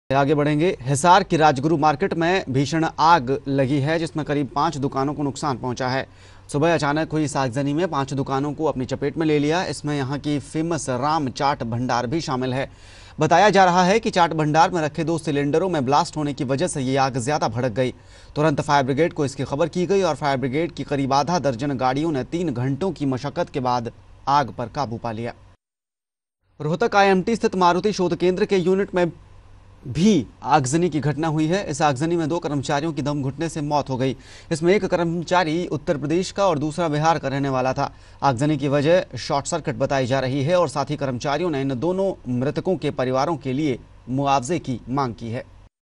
राजगुरु मार्केट में लगी भीषण आग
आगजनी में बाज़ार की पांच दुकानों का हुआ नुक्सान